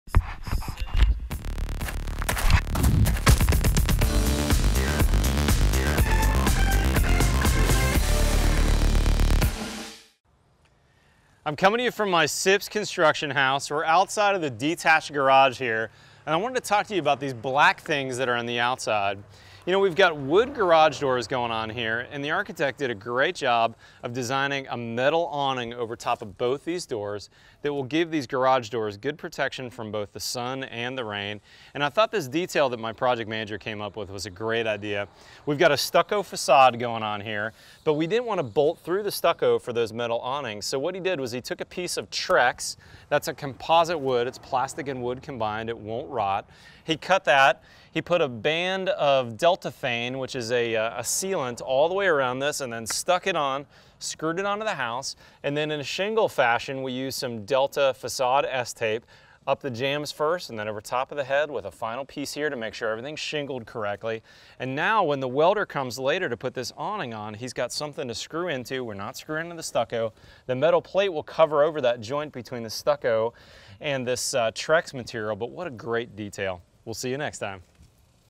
Flashing An Awning - SIPs House with Delta Vent SA
This SIPs construction house has metal awning above the windows/doors and a stucco facade.  In this video I'll show you how we flashed the supports.
Matt Risinger - Risinger Homes in Austin, TX